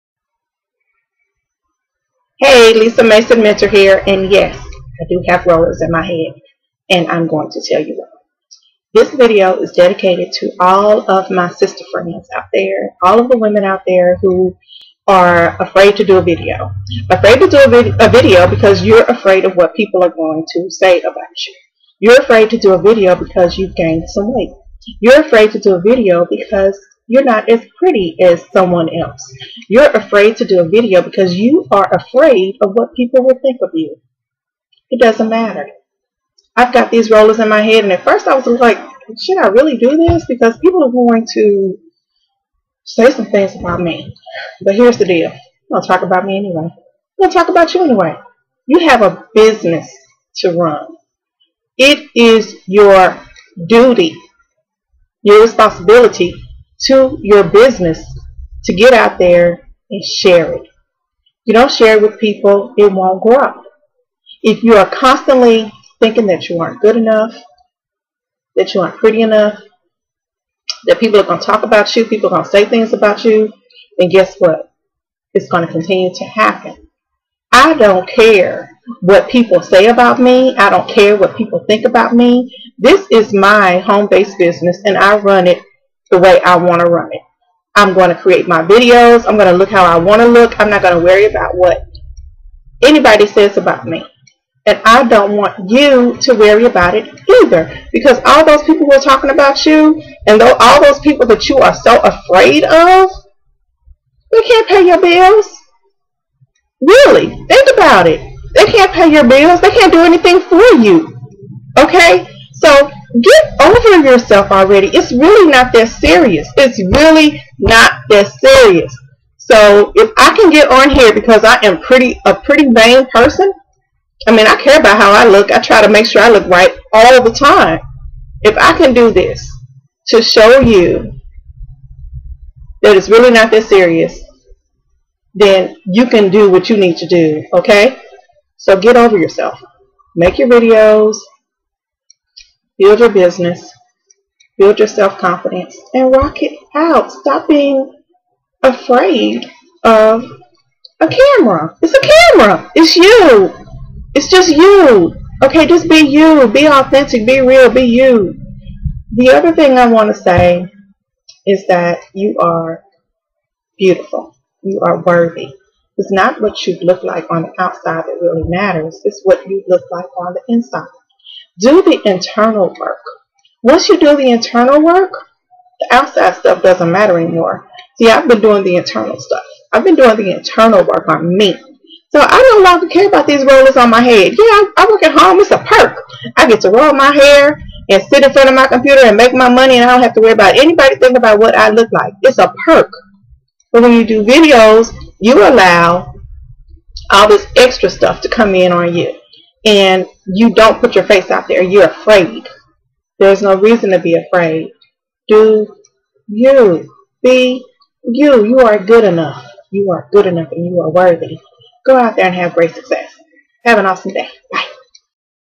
Overcoming Fear When You Make Videos
Lisa Minter shares overcoming fear when you make videos and why it's important to get over your fear of making videos, not caring what other people think and why you need to go ahead and step out of your comfort zone and make your videos. Your MLM success and network marketing business depends on exposure.